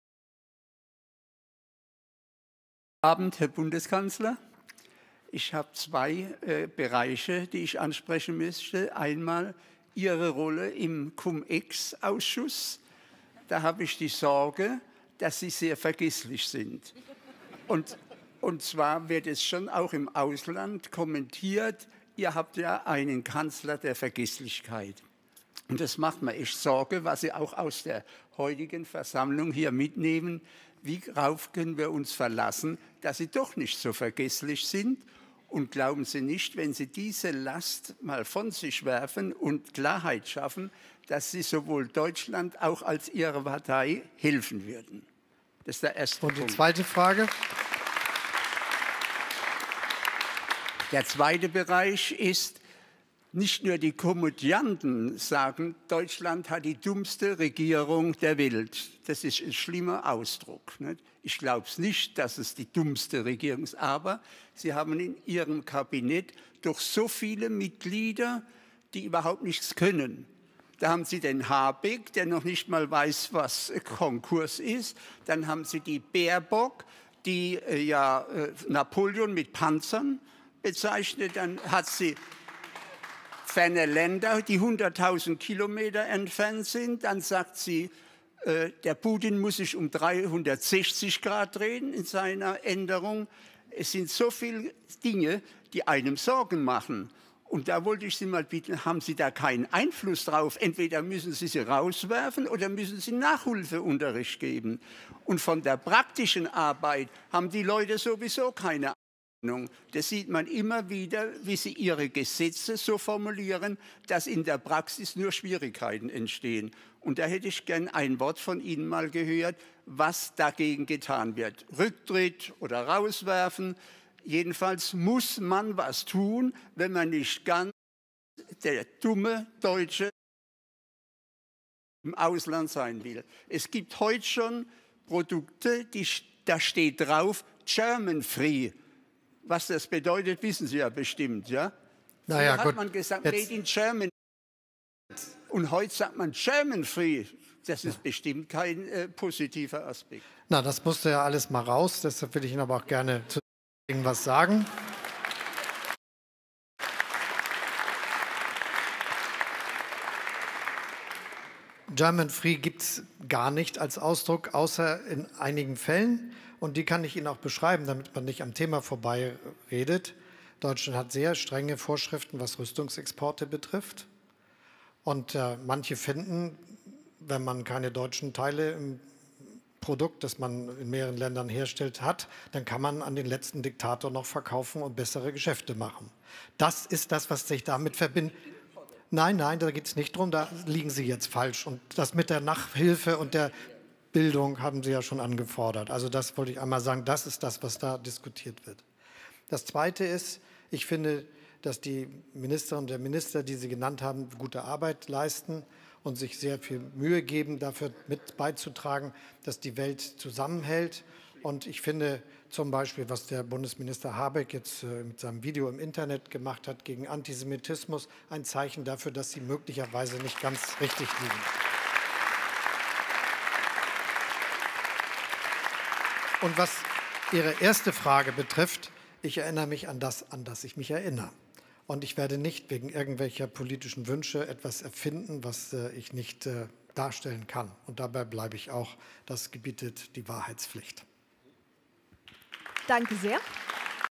Deutschland hat die dümmste Regierung der Welt. Das Bundesverdienstkreuz für diesen Mann‼️Scholz
Das Bundesverdienstkreuz für diesen Mann‼️ Video mit Antwort von Scholz 😂😂😂 Bitte verbreiten‼️

Bitte dabei „Geschenk“ angeben.